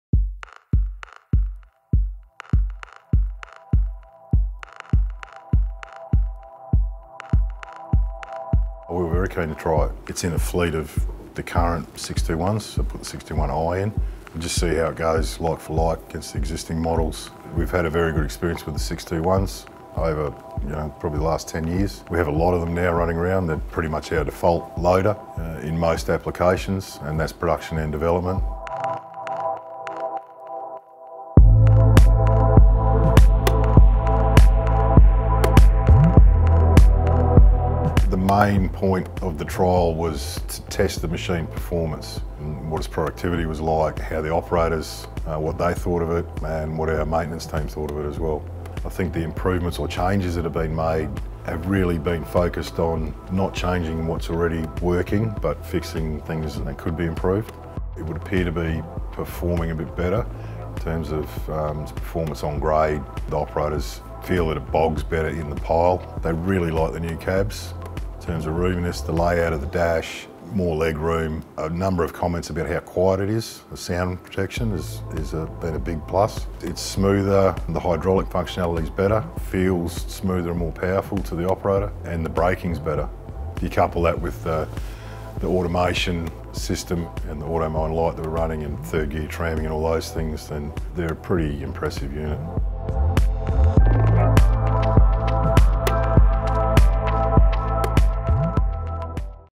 Byrnecut Australia trials the New Sandvik LH621i | Sandvik Mining and Rock Technology
Byrnecut Australia trialed our new Sandvik LH621i loader. Managing director Pat Boniwell praised the loader’s improvements, including performance on grade, cabin enhancements, hydraulic functionality and braking.